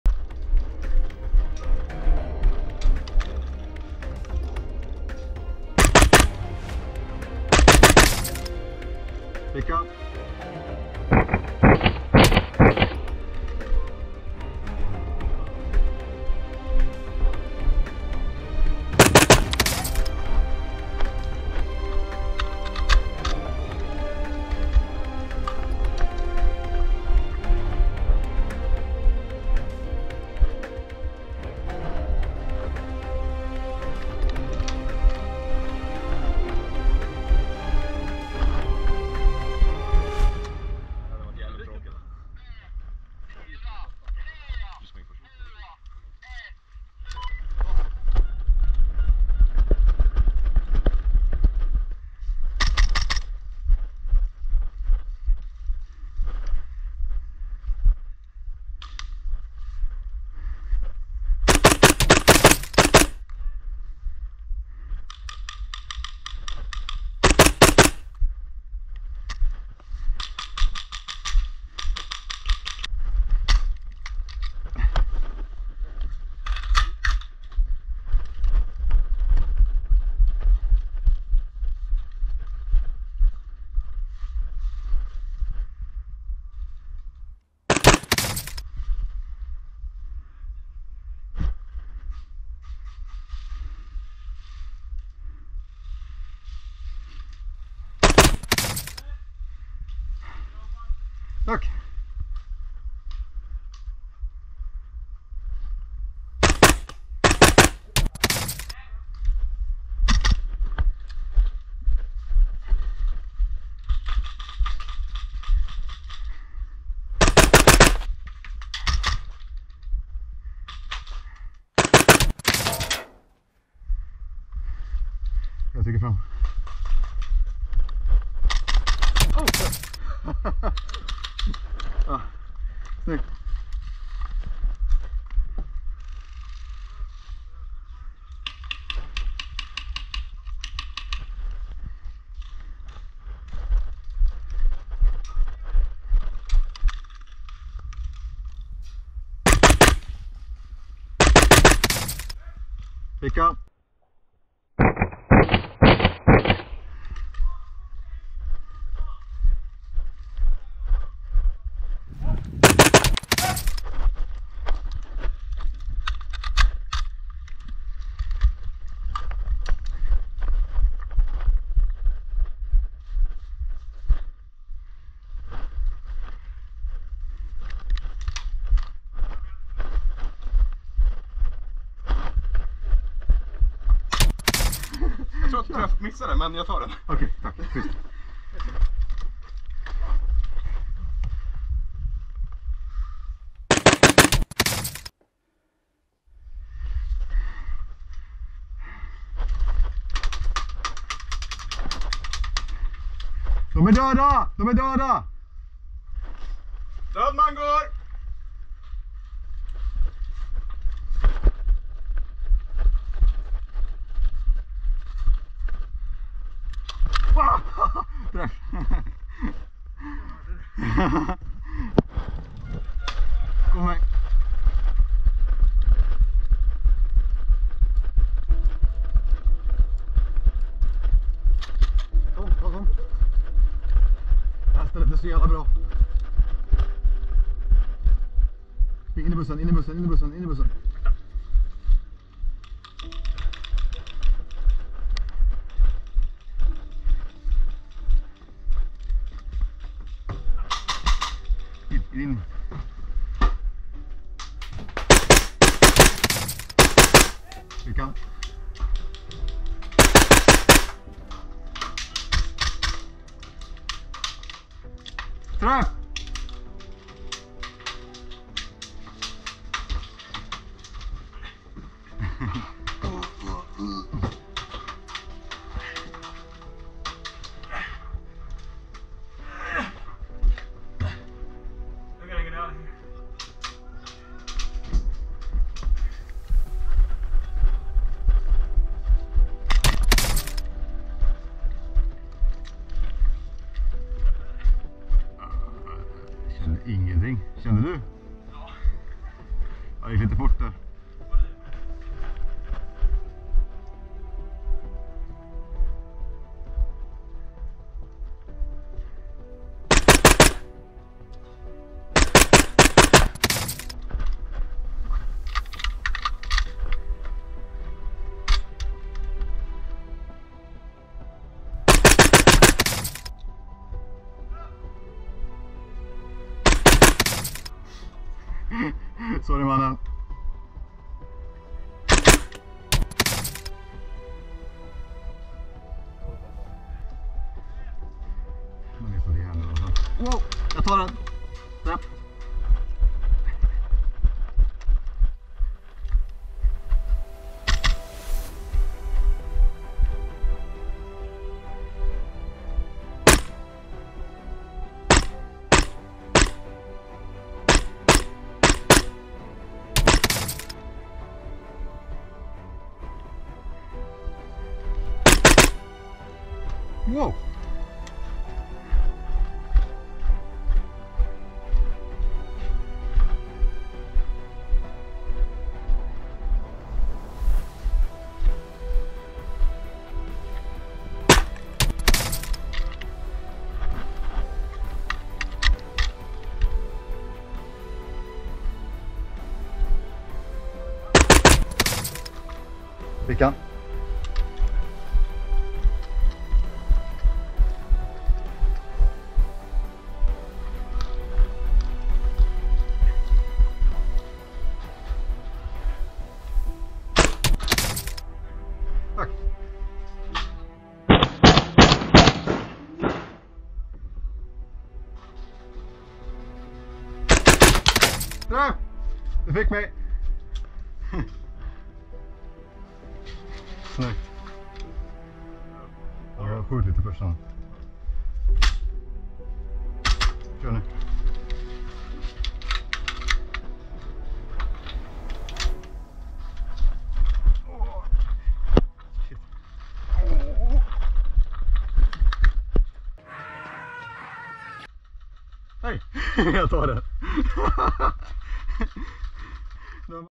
Airsoft - Blood Diamond GBBR CAR-15 Gameplay (AC Paintball)
This is my built from scratch TM MWS which features a NOVA A1 reciever set and GM/G&P internals.